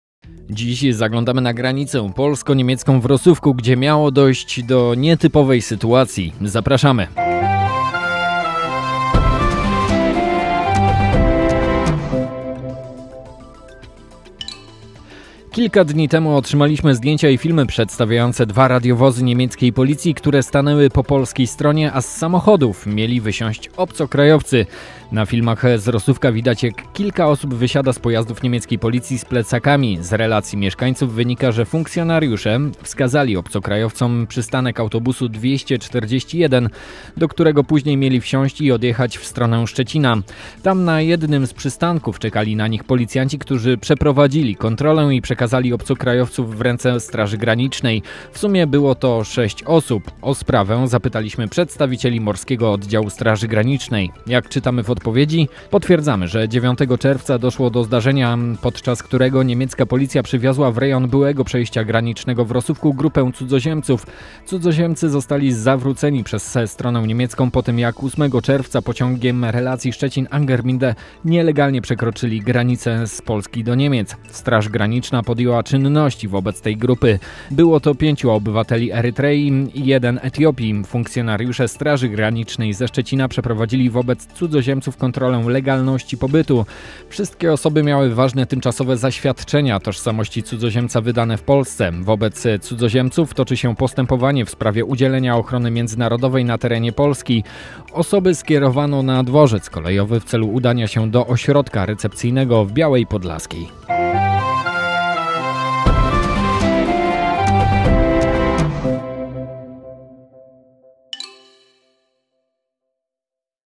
Nietypowe zdarzenia na granicy w Rosówku
Kilka dni temu otrzymaliśmy zdjęcia i filmy przedstawiające dwa radiowozy niemieckiej policji, które stanęły po polskiej stronie, a z samochodów mieli wysiąść obcokrajowcy. Na filmach z Rosówka widać jak kilka osób wysiada z pojazdów niemieckiej policji z plecakami.